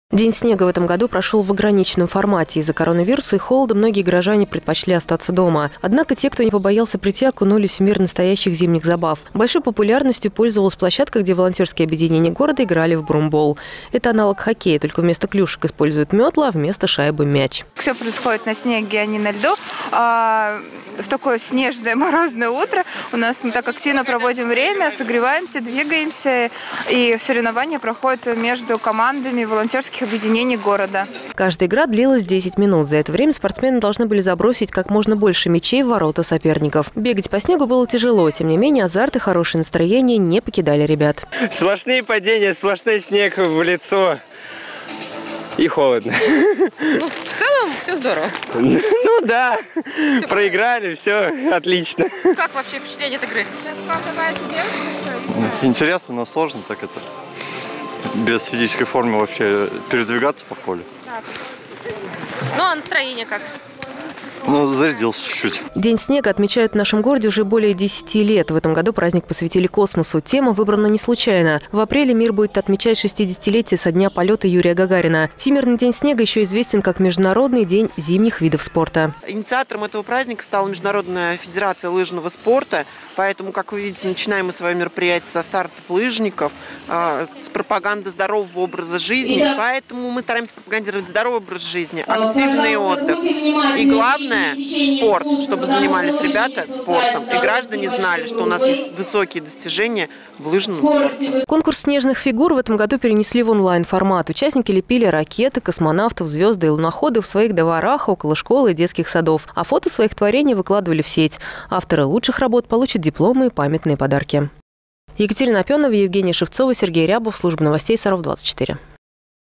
День снега
В воскресенье на Лыжной базе отметили День снега. Из-за пандемии коронавируса праздник прошёл в ограниченном формате.